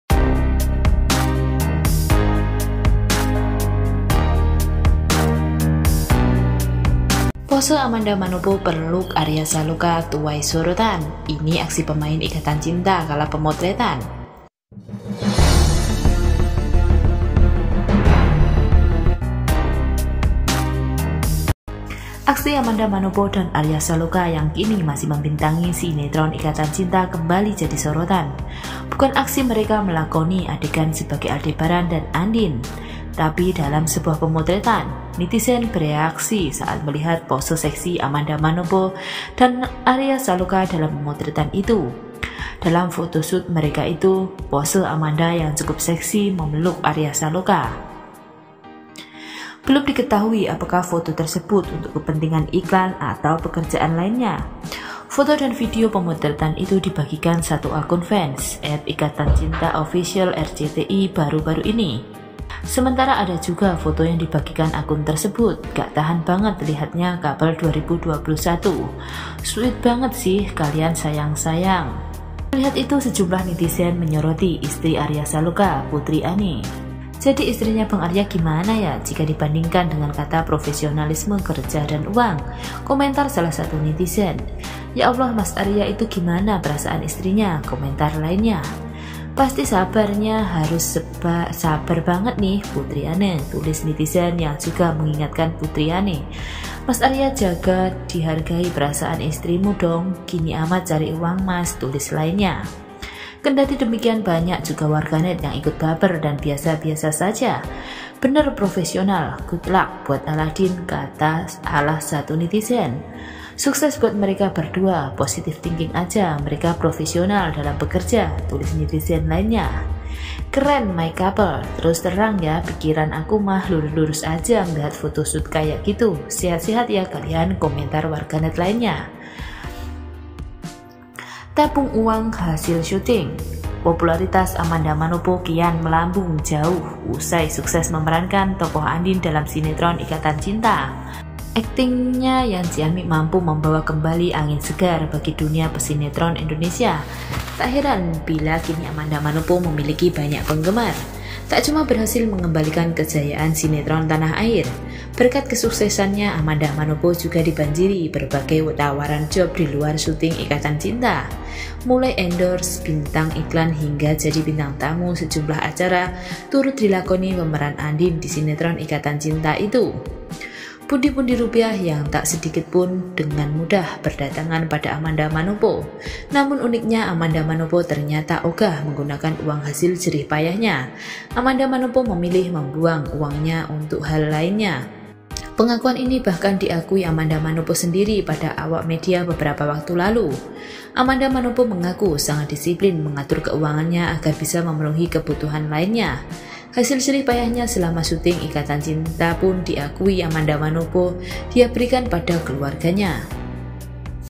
Pose Amanda Manopo Peluk Arya Saloka Tuai Sorotan, Ini Aksi Pemain Ikatan Cinta Kala Pemotretan
Aksi Amanda Manopo dan Arya Saloka yang kini masih membintangi Sinetron Ikatan Cinta kembali jadi sorotan.

Bukan aksi mereka melakoni adegan sebagai Aldebaran dan Andin, tapi dalam sebuah pemotretan.

Netizen bereaksi saat melihat pose seksi Amanda Manopo dan Arya Saloka dalam pemotretan itu.

Dalam photoshoot mereka itu, pose Amanda yang cukup seksi memeluk Arya Saloka.